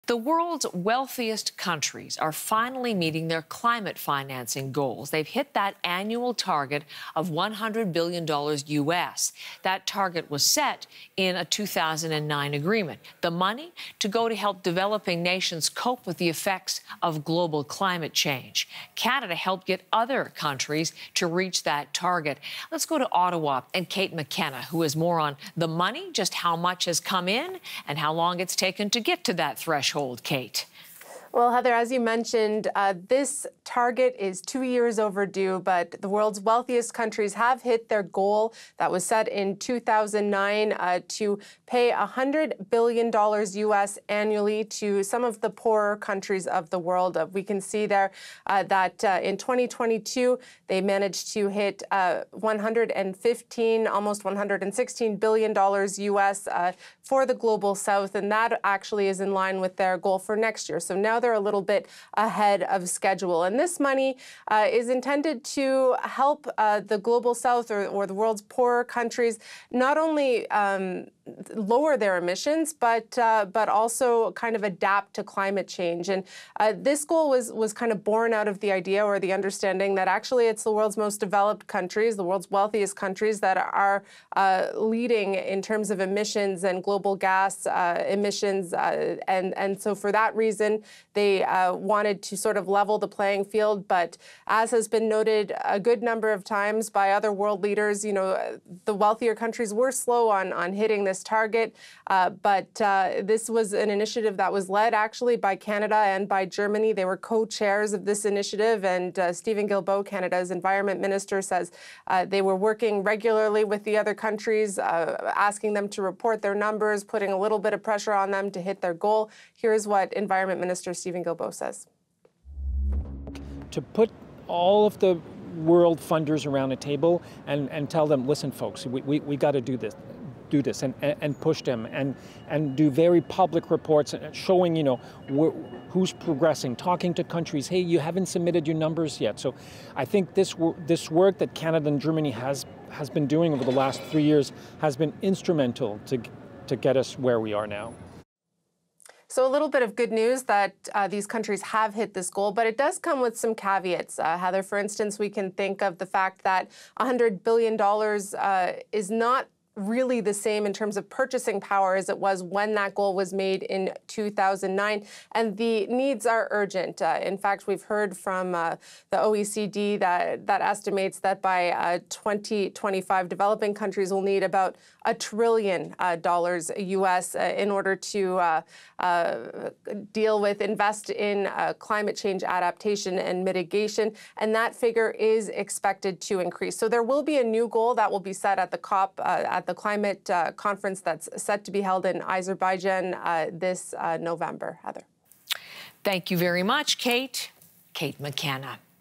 Rich countries hit $100B US climate finance goal — 2 years late
Canada has announced that wealthy countries have finally reached their goal of providing $100 billion US to help poorer countries both combat and adapt to climate change.